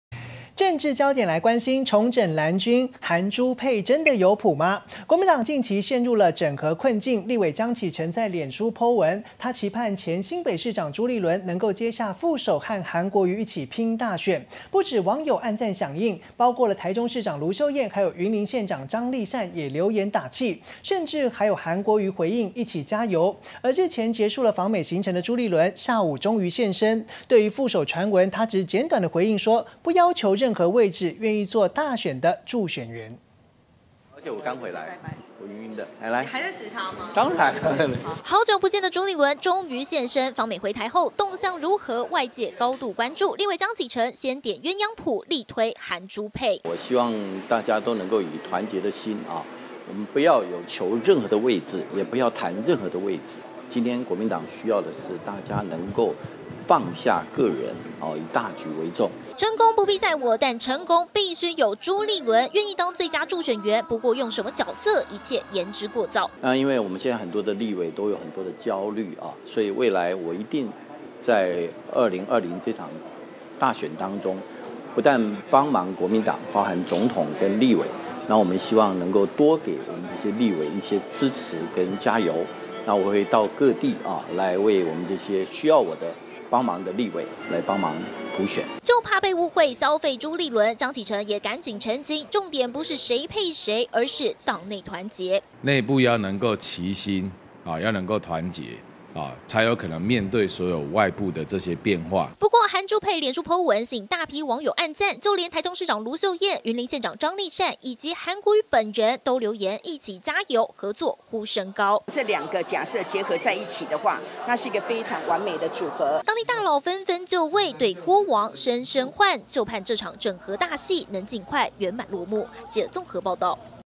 樂當"最佳助攻王" 朱立倫:成功必須有我
「韓朱配」真的有譜嗎？國民黨近期陷入整合困境，立委江啟臣在臉書PO文，期盼前新北市長朱立倫，能接下副手與韓國瑜拚大選，不只網友按讚響應，台中市長盧秀燕、雲林縣長張麗善，也留言打氣，甚至還釣出韓國瑜回應「 一起加油！」，而日前結束訪美行程的朱立倫，今天下午(30號)也終於現身，對於副手傳聞，只簡短回應，「不要求任何位置，願意做大選助選員」。